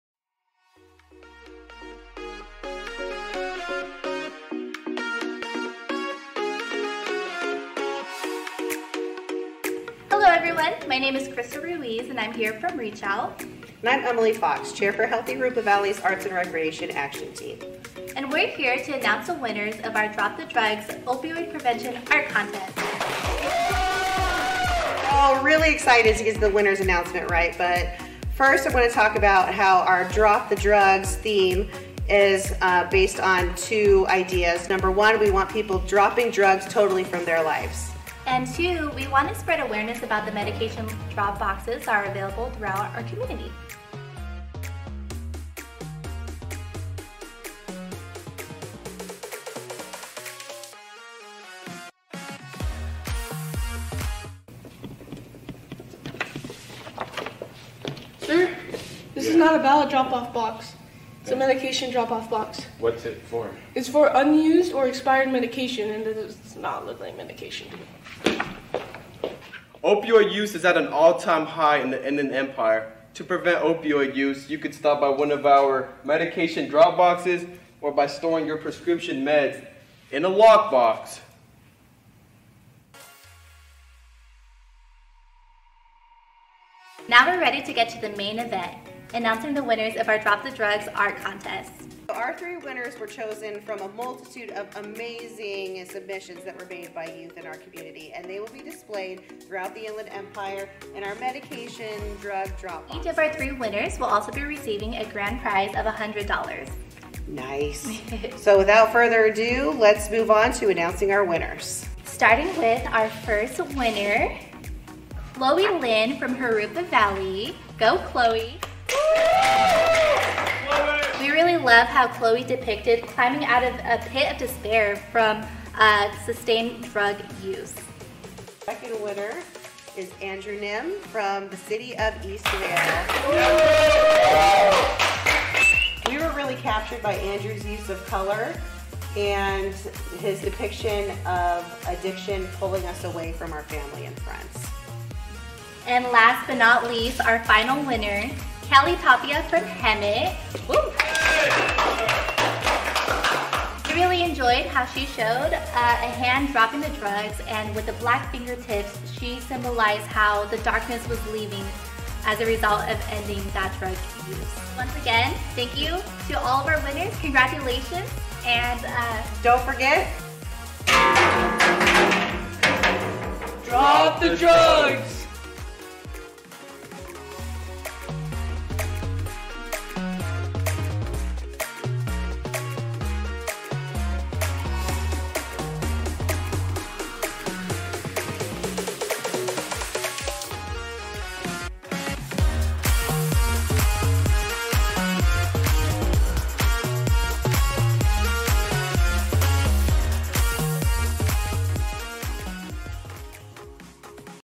Drop the Drugs Art Contest Winners Reveal Video
“Drop the Drugs” - Opioid Prevention Art Contest

The misuse of opioid pain medication—even just one time!—can lead to serious illness or death. In 2016, 1 in 5 deaths among young adults were opioid-related. These numbers are only getting worse. Overdose deaths linked to synthetic opioids like fentanyl tripled among teenagers and went up five times among Black teens in the past two years while suicides by drug overdose increased among young people in 2020 (CDC, 2021).

We asked for your help bringing this crisis to an end to save the lives of our young people. Studies have shown that opioids found in our homes and medicine cabinets have become the supply of choice for young people. Prescription Drug Drop Boxes give communities a safe way to dispose of their medications.

3 Winning entries will have their artwork displayed on a Prescription Drug Drop Box, be featured in other places throughout the community, and on social media!

Congratulation to our 3 winners!